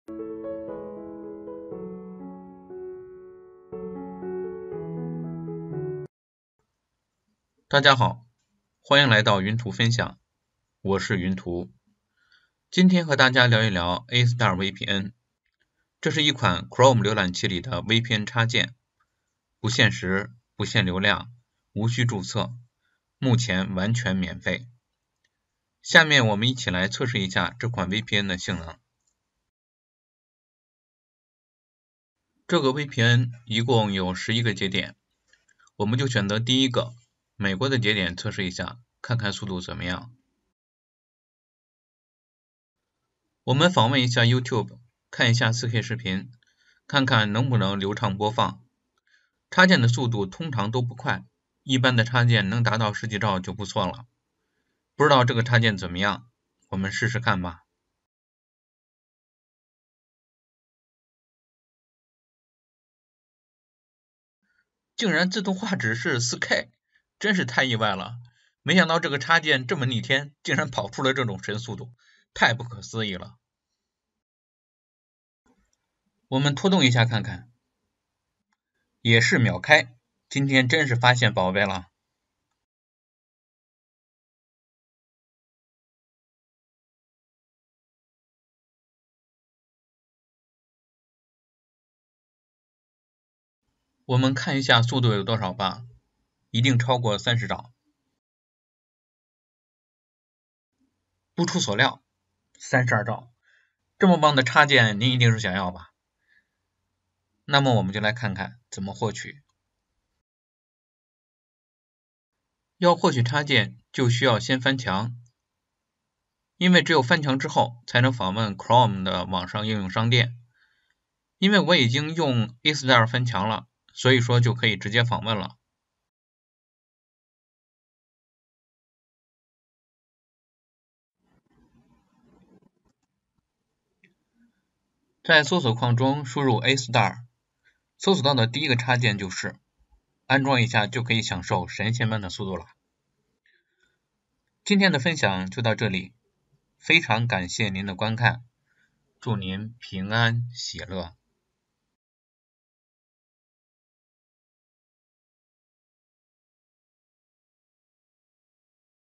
【翻墙软件】分享一款Chrome VPN插件ASTAR，自动画质4K，免费使用
分享一款Chrome VPN插件，自动画质4K，神仙速度，免费使用